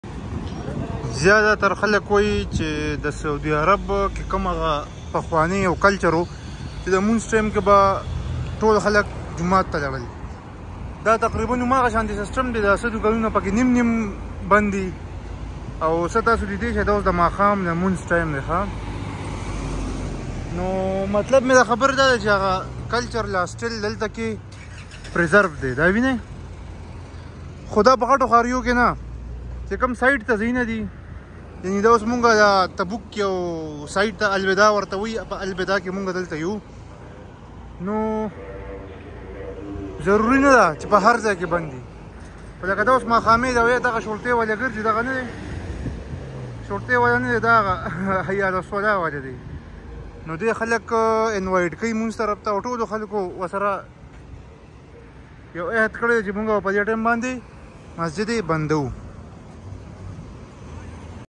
Life in Saudi Arabia Albada life in tabuk ksa vlogs saudi vlogs in ksa tabuk streets old albada
Shops are closed at the time of magrib prayer in saudi arabia
Life in saudi arabia still has the old tradition in its towns